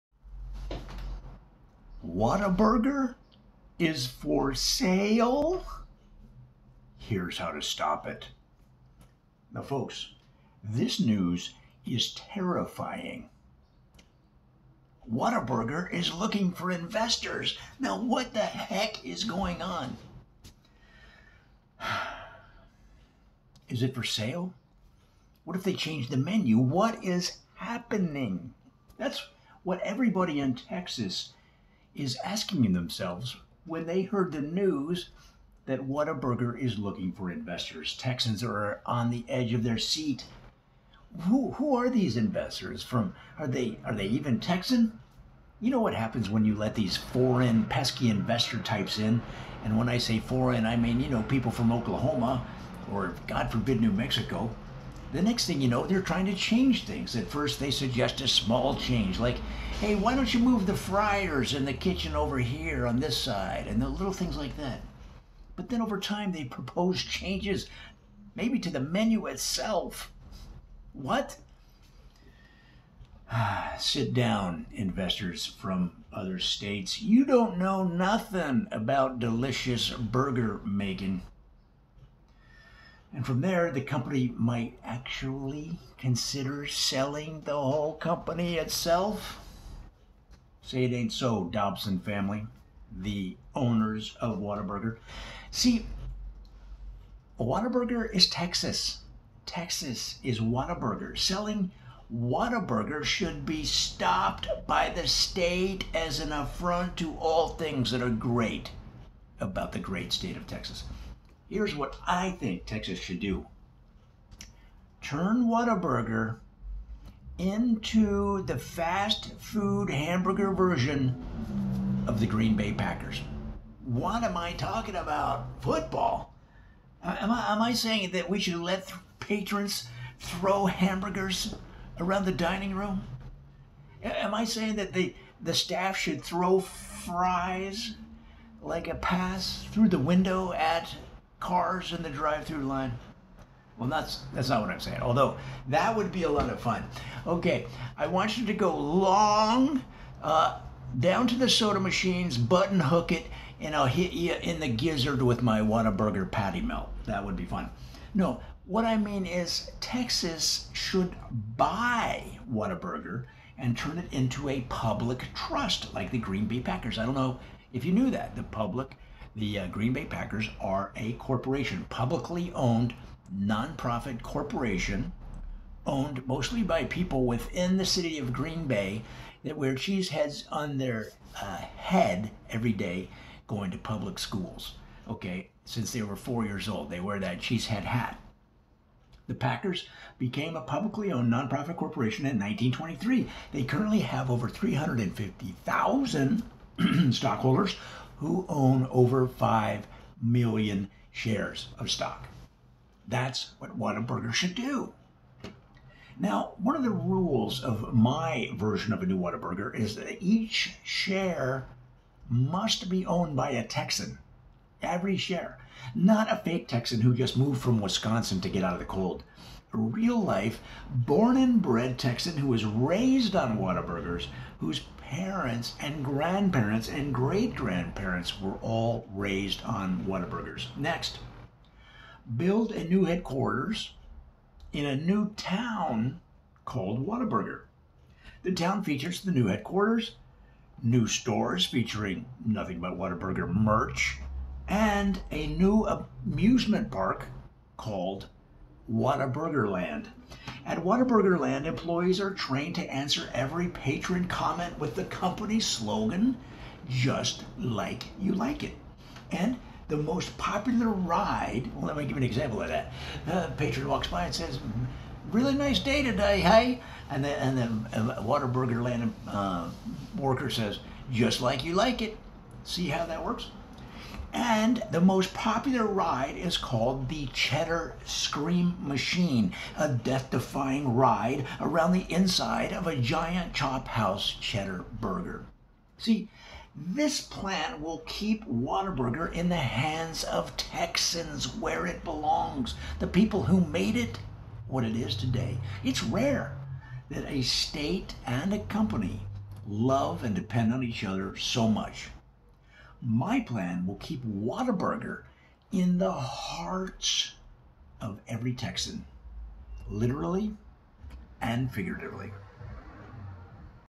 Whataburger is For Sale ? Here's How to Stop It
Warning: This news is terrifying.

Whataburger is looking for investors.

Is it for sale? What if they change the menu? What is haaaaapppeeninnng?

Texans are on the edge of their seat with the news that Whatburger is seeking investors to fund expansion. You know what happens when you let these pesky investor-types in. The next thing you know, they are trying to change things.

At first, they suggest a small change like how the fryers should be arranged in the kitchen. Over time, they propose changes to the menu itself. Forget it.

Sit down, investors from other states. You don’t know nothing about no delicious burger-making!

And from there, the company might actually consider SELLING THE CHAIN ITSELF. What? Say it ain’t so, Dobson family.

Selling Whataburger should be stopped by the state as an affront to all things that are great about Texas.

Here’s what Texas should do: Turn Whataburger into the hamburger version of the Green Bay Packers.

What? Let patrons throw hamburgers around the dining room like footballs? Throw fries threw the window at the drive-through? No, although that would be a lot of fun: “OK, I want you to go long around the far tables by the soda machine. Button hook and I’ll hit you in the gizzard with my Whataburger Patty Melt!”

No, what I mean is Texas should BUY Whataburger and turn the company into a public trust like the Green Bay Packers. The Packers became a publicly-owned, non-profit corporation in 1923. They currently have over 350,000 stockholders who own over 5 million shares of stock.

One of the rules of my version of a new Whataburger is that each share must be owned by a Texan. Not a fake Texan who just moved from Wisconsin to get out of the cold. A real life, born and bred Texan who was raised on Whataburgers, whose parents and grandparents and great grand parents were all raised on Whataburgers.

Next, build a new headquarters in a new town called Whataburger, Texas. The town features the new HQ, new stores selling nothing but Whataburger merch, and a new amusement park called Whataburgerland.

At Whataburgerland, employees are trained to answer every patron comment with the company slogan, “Just Like You Like It.” And the most popular ride is called the Cheddar Scream Machine, a death-defying ride around the inside of a giant Chop House Cheddar Burger.

This plan will keep Whataburger in the hands of Texans, the people who made it what it is today. It’s rare that a state and a company love and depend on each other so much–my plan will keep Whataburger in the heart of every Texan. Figuratively and literally.

XXXXXXXXXXXXXXXXXXXXXXXXXXXXXXXXXXXXXXXXXXX
Joe Ditzel is a humor writer, comedian and really bad golfer.